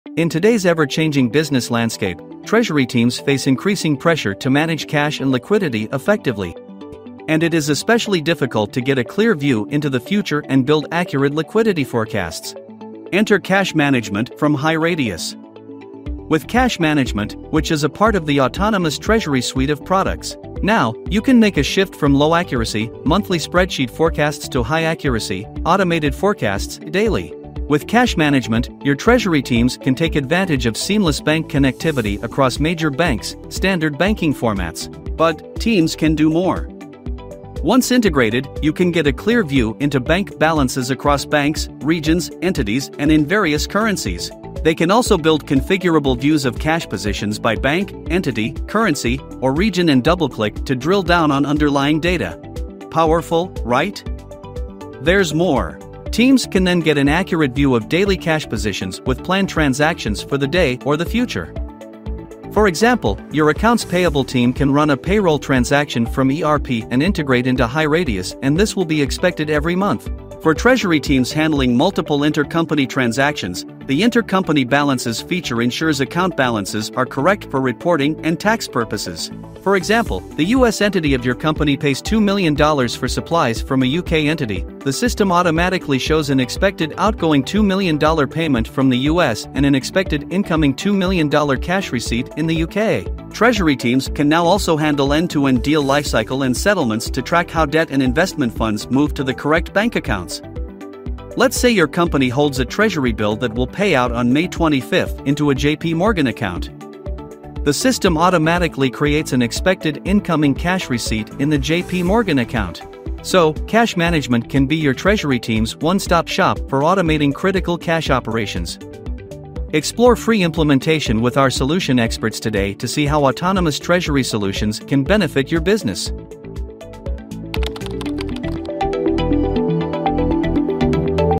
Get Complete Control over Your Cash with Cash Management Software | HighRadius
With HighRadius Cash Management Software, view, manage, analyze, report, forecast- Get complete control over your cash without the guesswork, hassle, and complexity.

HighRadius Cash Management Software is a secure, easy-to-use SaaS platform capable of automatically retrieving your Bank and other transaction data and reconciling and encoding it on a daily basis. The Cash Position Worksheet within the software is a flexible and intuitive tool that will replace your spreadsheet-based manual processes and enable unlimited combinations of views by bank, currency, business unit, or region, on-demand.

Moreover, the transaction database makes historical reporting robust and easy to use. With the HighRadius Cash Management module, treasury users can free up considerable time, reduce manual errors, and have confidence in the reliability of their processes.

HighRadius Cash Management Software helps businesses:

1. Automate the bank data collection and gathering process
2. Automate the Bank Reconciliation process after import
3. Automatically generate Cash Position worksheets using reconciled prior-day activity plus current-day reports
4. Optimize borrowing or investment needs based on your target balances
5. Schedule or run on-demand cash management activity reports with robust reporting capabilities.

Trusted by the fastest-growing global companies in the industry, the HighRadius Cash management solution helps to view global cash, track cash positions, and manage debt and investment deals- all in one place.
Key Moments:
0:00 - 0:12: Challenges Faced by Treasury Teams
0:12- 0:35: HighRadius Cash Management Software
0:36-1:53: Features of HighRadius Cash Management Software
1:54- 2:11: Conclusion